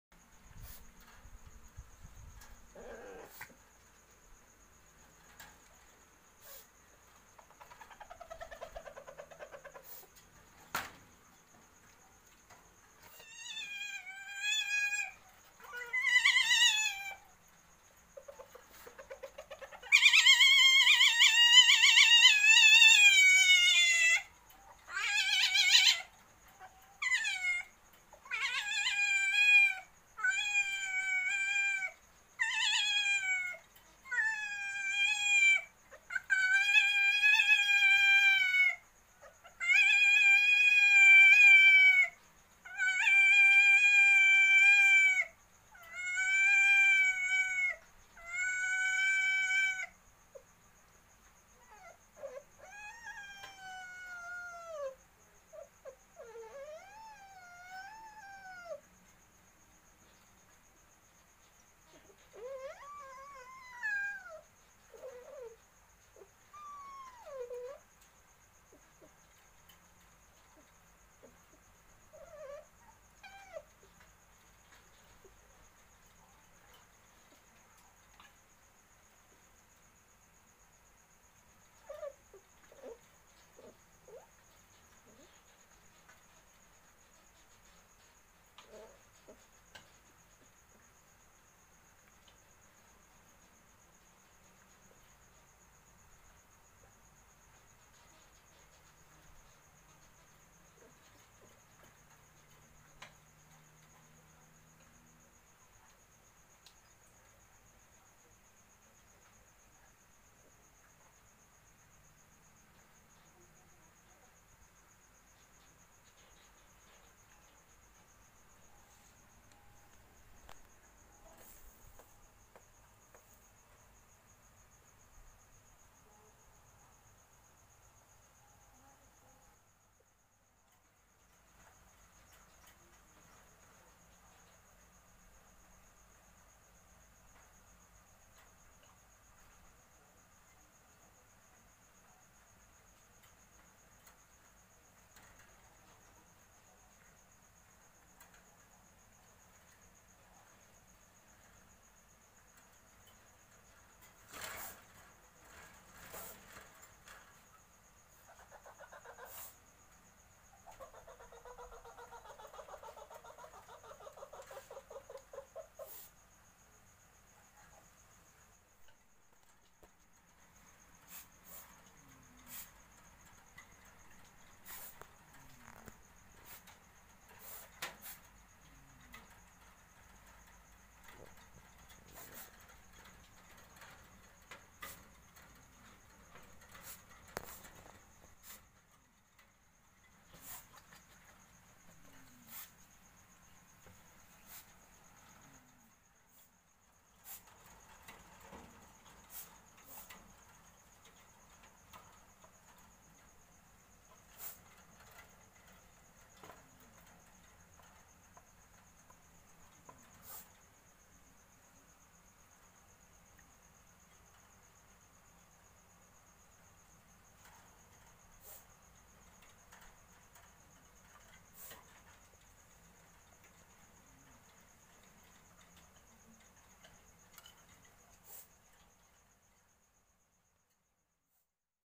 🔴 CARA MUDAH BRANDING MUSANG PANDAN.
cara mudah dan simpel branding musang pandan agar cepat beranak.